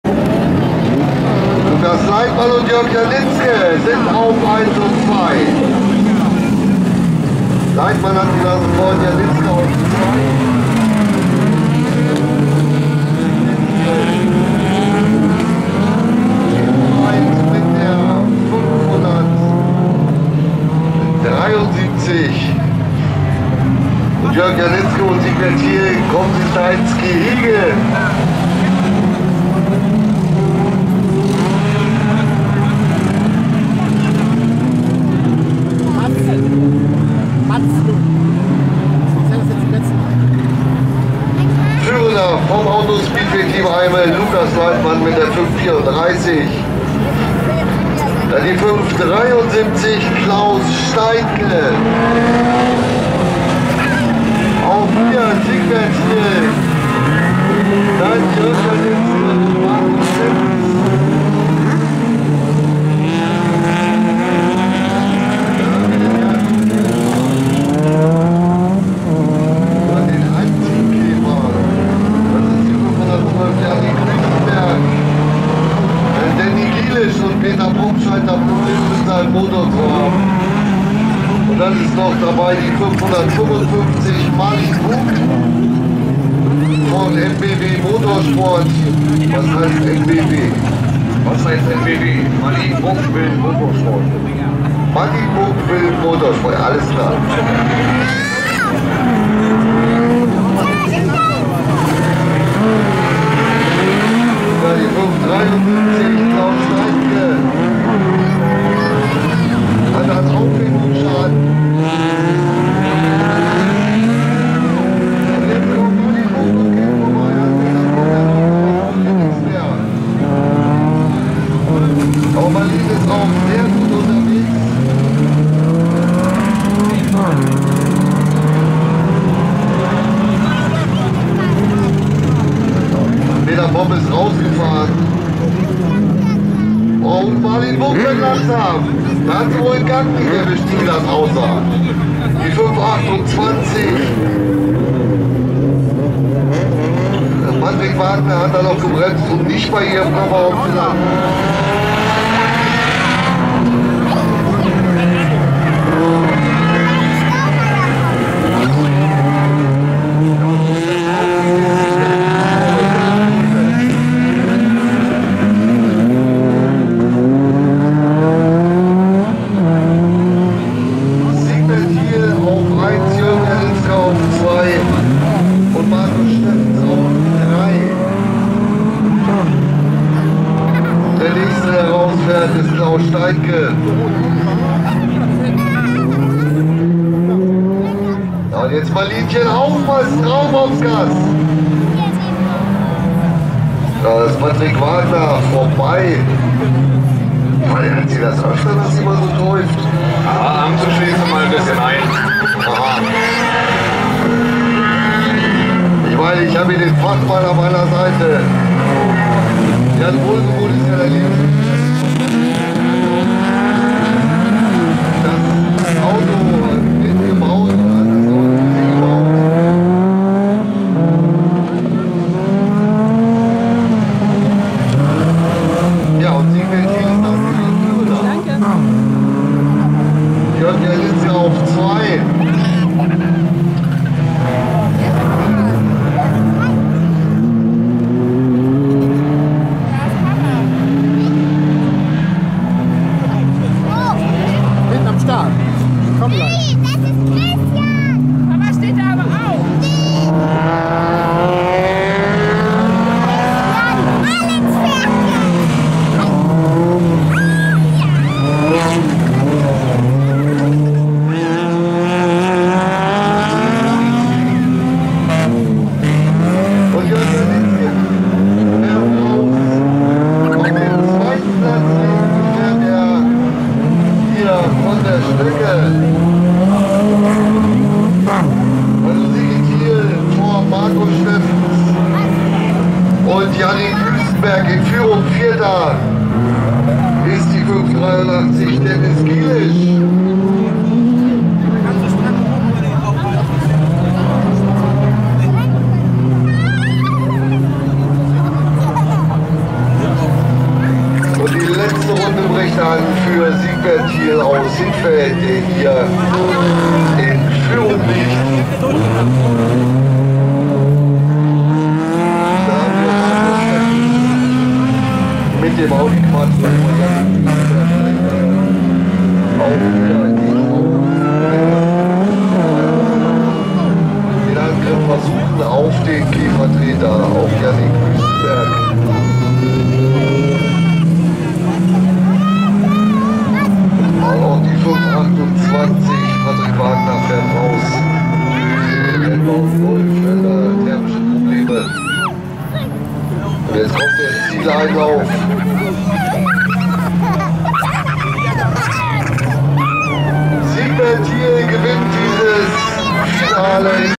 Finale der Klasse 5 - Birkenring in Hoope - 27.10.2019
Jubiläumsveranstaltung - 60 Jahre  Motor Club "Blaue Jungs" Hellingst seit 1959 auf dem Birkenring in Hoope bei Bremen.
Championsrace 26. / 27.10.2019